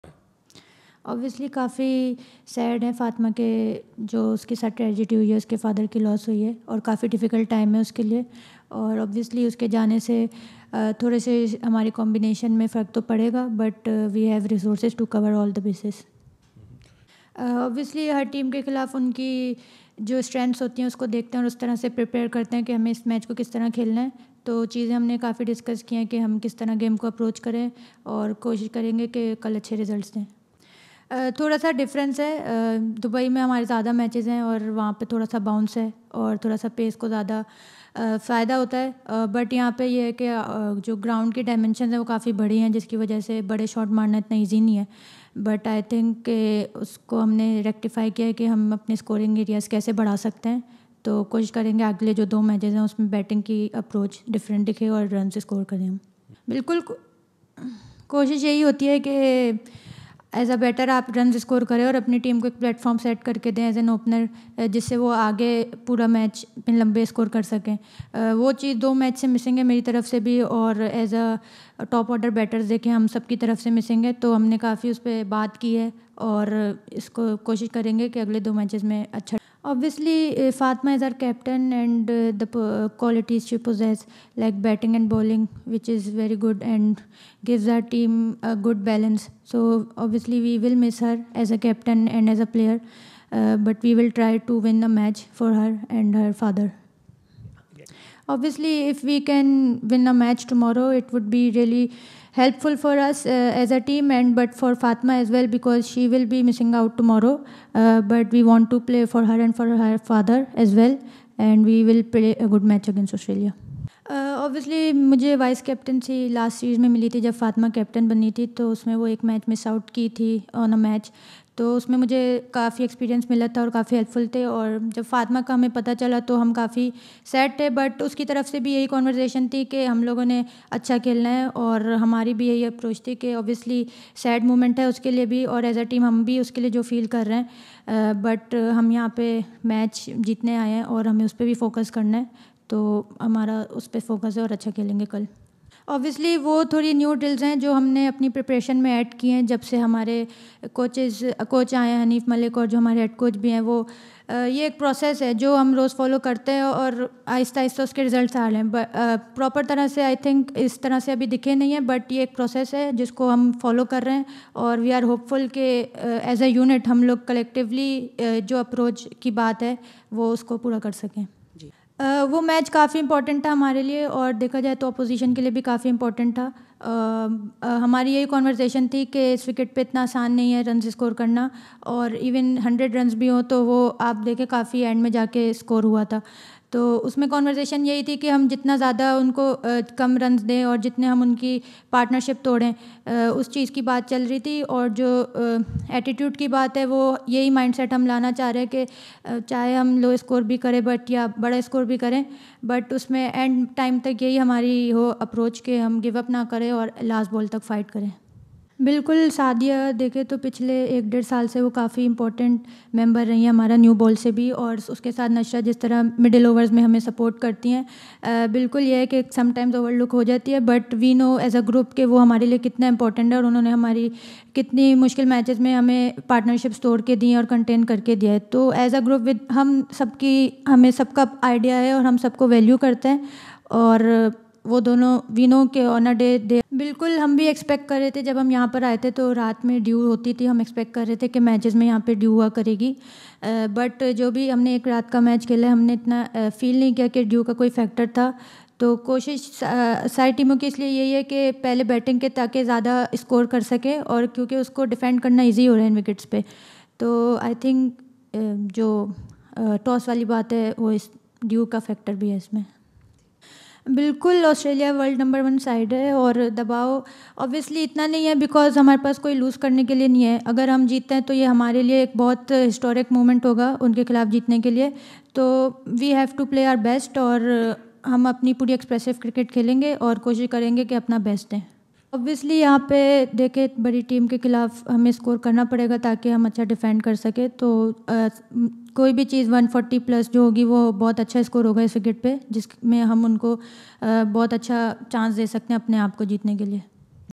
Muneeba Ali Press Conference
منیبہ علی کو پاکستان کرکٹ ٹیم کا کپتان راتوں رات کیوں بنا دیا گیا گیا ؟